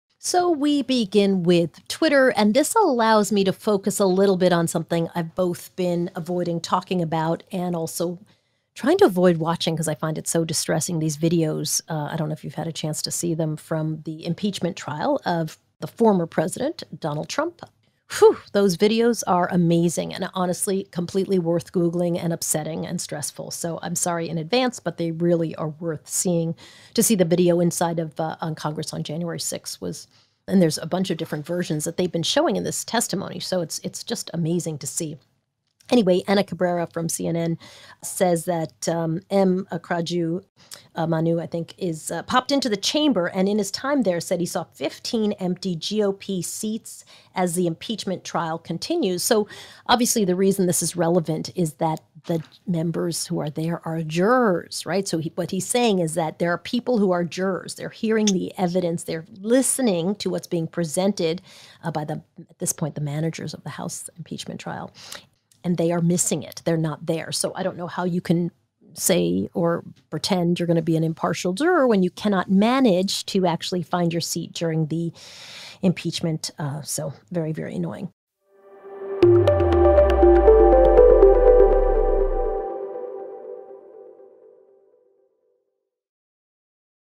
Soledad O'Brien talks Trump's impeachment trial
Did you watch the insurrection footage presented during the trial?